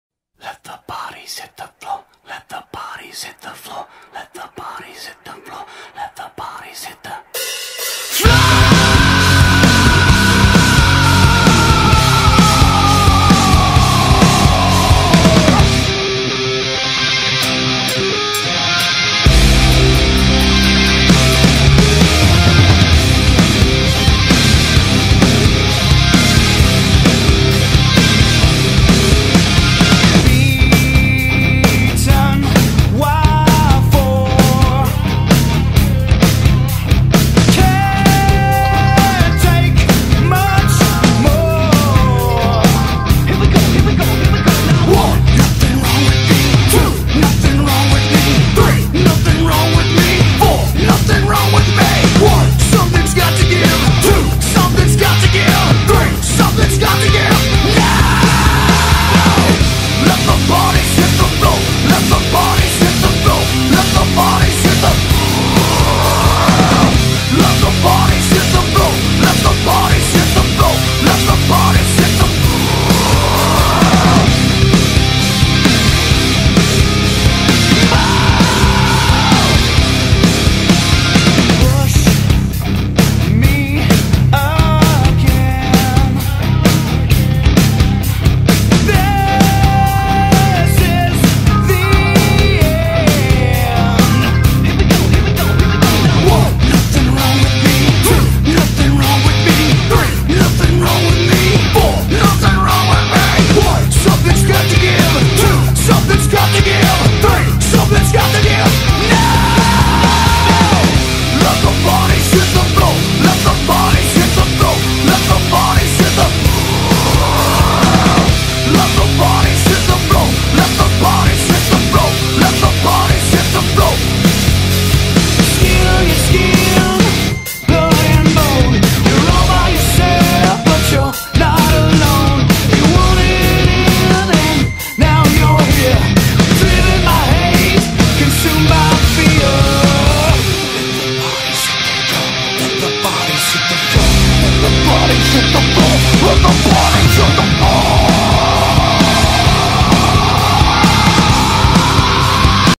911Signal&Safety custom conversion led Light bar
This is a Highly customized bar for a Highly customized fire dept response unit.Using the Atom processor for additional patterns and auto adjustable to lighting conditions,variable wattage from .05 to 1.2 watts.External solar connection for ignition off operation.Unit has an additional 4 patterns randomized per clients request.This is a common lightbar with uncommon features.Over 880 leds involved.And 16hrs labor,Unit sold for more than 3k.